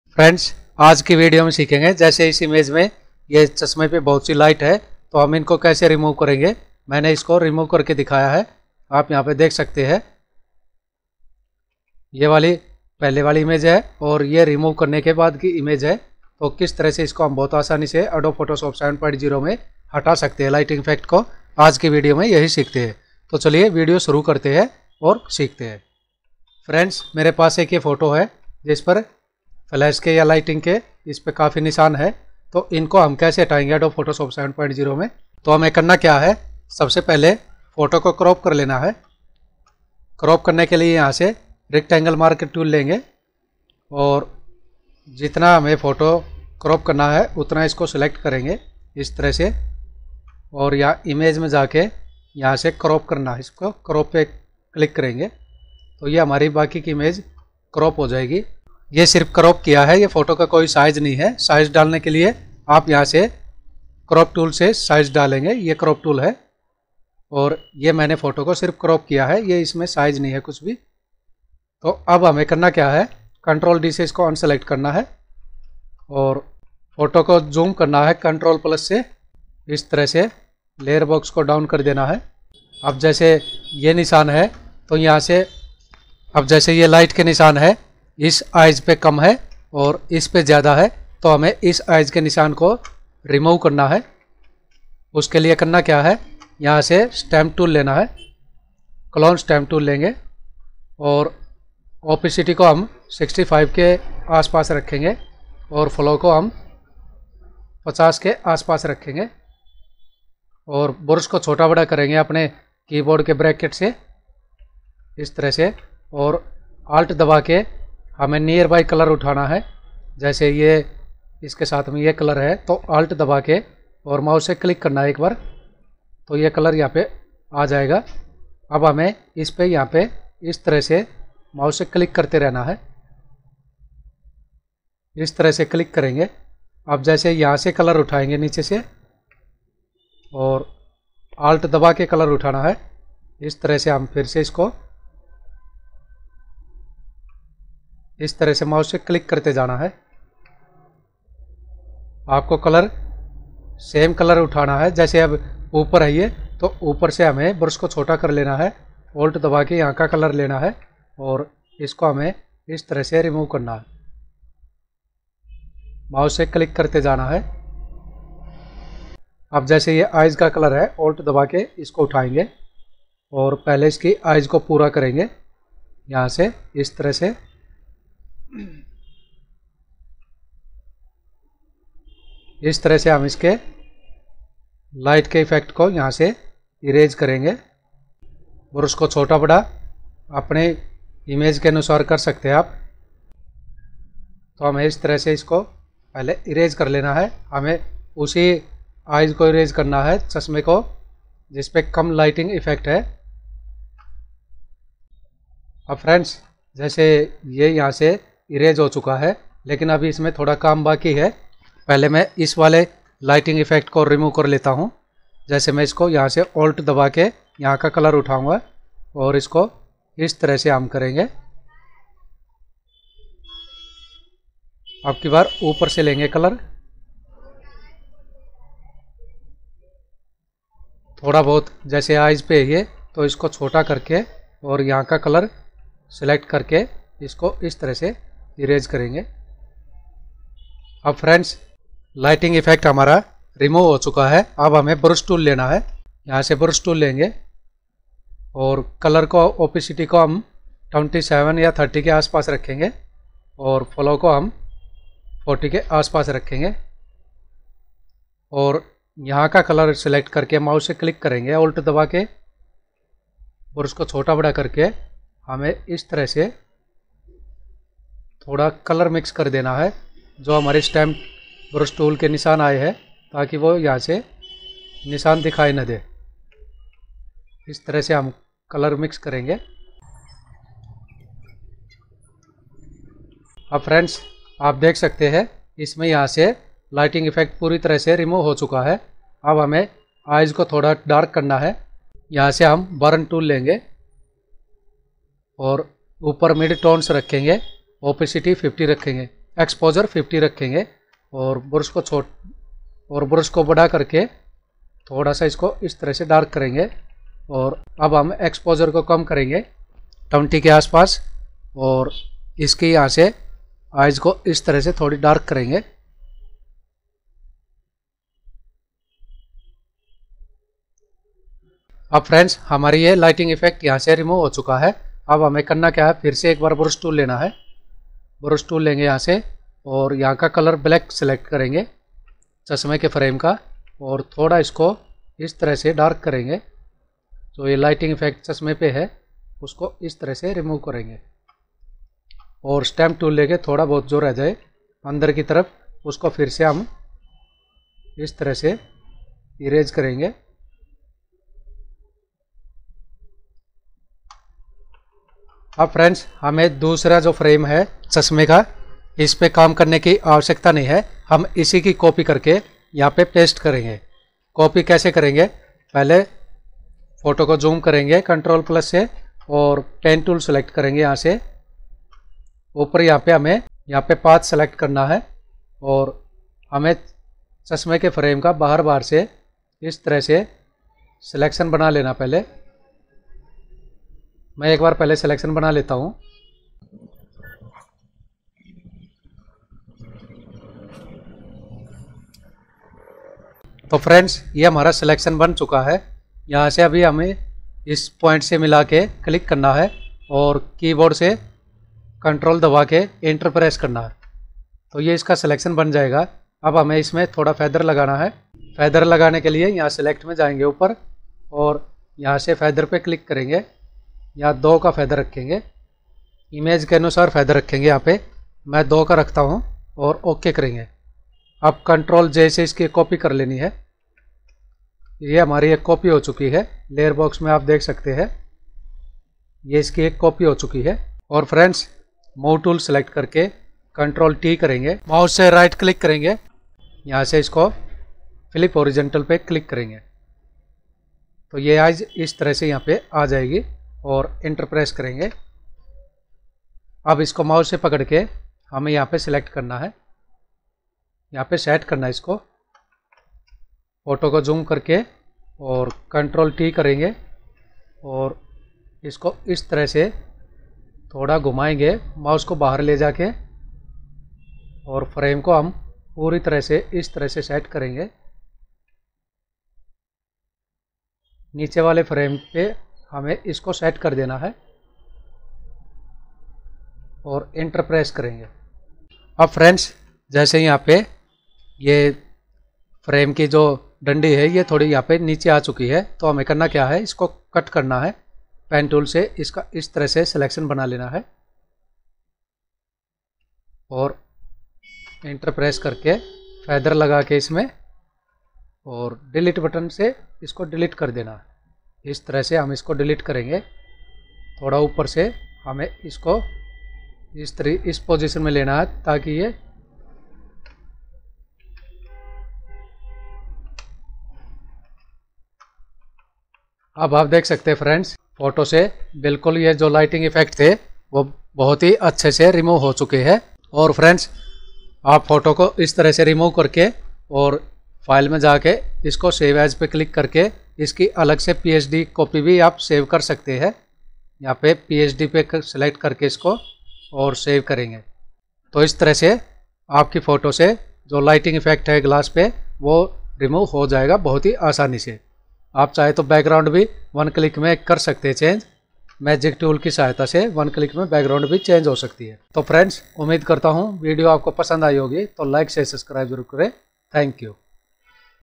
Magically Remove Glare from Glasses in Photoshop! फ़ोटोशॉप में चश्मे से चकाचौंध हटाएं! Photoshop
Photoshop cc 2022  (New Channel)

Video about:
Photoshop me chashme se light kaise hataye
photoshop me chasme ki lighting kaise hataye
aj is video se sikhange.